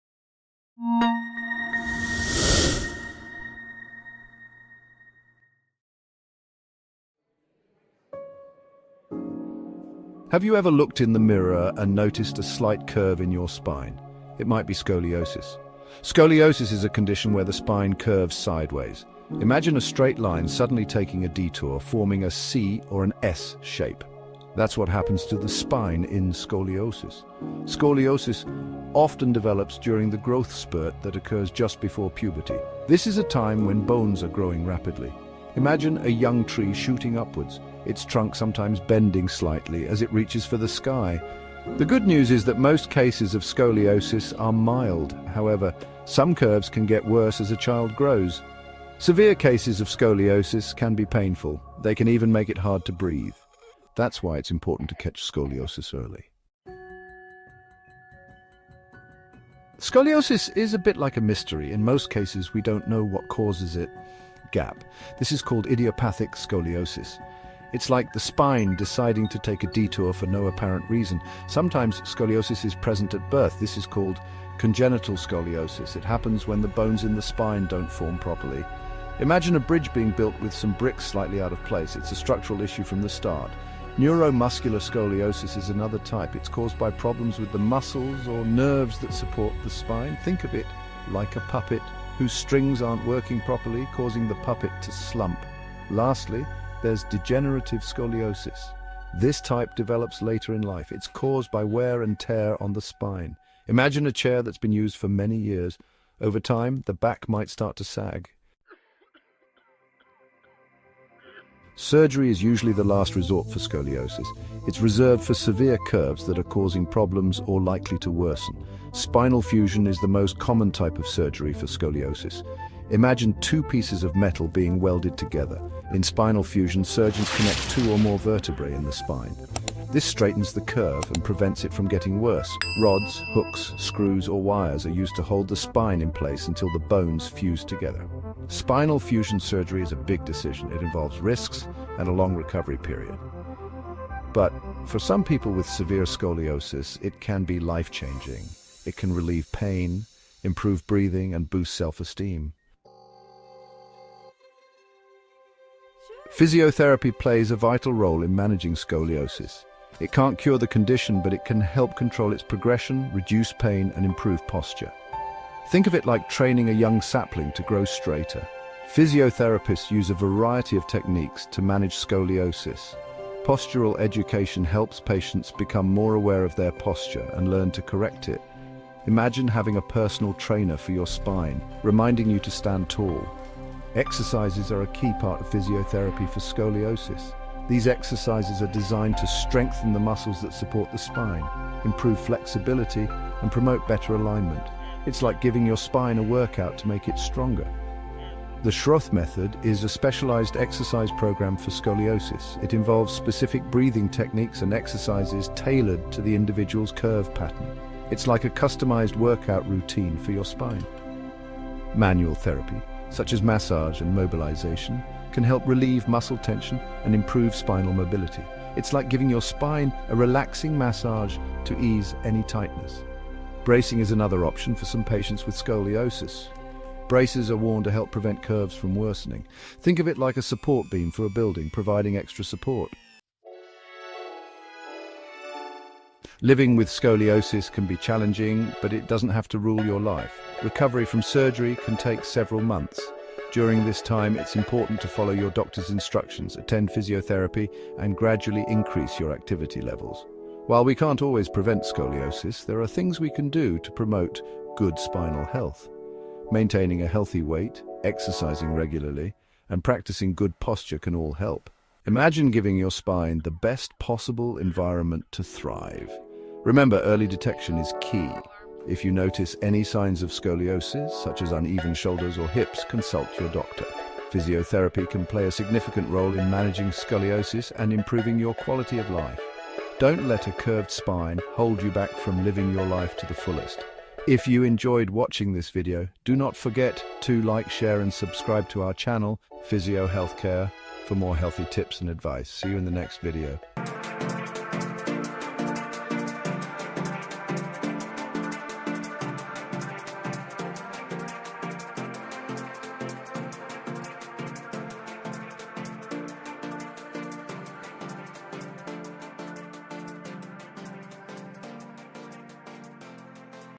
What Is Scoliosis? Scoliosis Surgery Vs Non-Surgical Physiotherapy Management Explained
What Is Scoliosis? Scoliosis Surgery and Non-Surgical scoliosis Explained by Physio Healthcare.

🧐 Can’t get enough about What is Scoliosis? Confused if Scoliosis Surgery Treatment or Non-Surgical Scoliosis treatment would benefit you? This video has all the answers! 🌟 If you are looking for a slight curve in the spine, whether small or large and want to explore the best options for managing scoliosis, this video is for you. From types of scoliosis to effective solutions, Part I of this video will guide you. 💪

Congenital scoliosis and idiopathic scoliosis are two of the different types of scoliosis. Understanding the types of scoliosis is essential for proper diagnosis. In this video, we’ll explore the causes of scoliosis, along with Scoliosis Surgery and Non-Surgical scoliosis Explained

💡 What you'll learn in this video:

✔️ What is scoliosis? Discover the basics of this condition and how it impacts the spine in scoliosis patients.
✔️ Explore the most common type of scoliosis, including idiopathic scoliosis, degenerative scoliosis, neuromuscular scoliosis, and congenital scoliosis. Learn how these different types of scoliosis arise and affect the spine.
✔️ Detailed insights into Scoliosis Surgery, such as spinal fusion surgery, and when scoliosis surgery becomes necessary.
✔️ Effective Non-Surgical Scoliosis treatment options, including the Schroth method for scoliosis, bracing for scoliosis treatment, and physiotherapy management.
✔️ Learn breathing techniques and spinal mobilization exercises that can help manage scoliosis naturally.

🔥 Key topics covered include:
Scoliosis Explained: Understand the causes of scoliosis, symptoms, and available treatment methods.
Expert insights into surgery for the management of scoliosis, including deciding when scoliosis needs surgery.
A detailed look at the role of physio and physical therapists in providing exercises for scoliosis and techniques to improve quality of life.
Explore how bones growing rapidly can impact scoliosis progression.

🌟 Don't miss:
A step-by-step explanation of what is scoliosis surgery?
How the Schroth method scoliosis approach and bracing play a role in non-surgical scoliosis management.
The importance of physiotherapy treatment for scoliosis and tips for managing scoliosis naturally.

👉 This video provides a comprehensive guide to everything you need to know about scoliosis—from types of scoliosis explained to advanced scoliosis treatment options. Whether you're curious about whether you’re looking for information on mild scoliosis or severe scoliosis, you’ll enjoy reading reviews in this blog!
🔥 Moving from scoliosis causes, symptoms, types, and risk factors: Here is how to cope with scoliosis. Here are the best surgical and non-surgical Scoliosis treatments, such as spinal fusion, and Schroth exercises.

📌Timestamps

0:00 - Intro: Scoliosis Explain
0:50-1:41 What is scoliosis
1:42–2:26 What can be the problem for scoliosis
2:27–3:47 How to help physiotherapy to recover scoliosis
3:48–4:46  talking about Scoliosis surgery

What is scoliosis surgery?
When does scoliosis need surgery?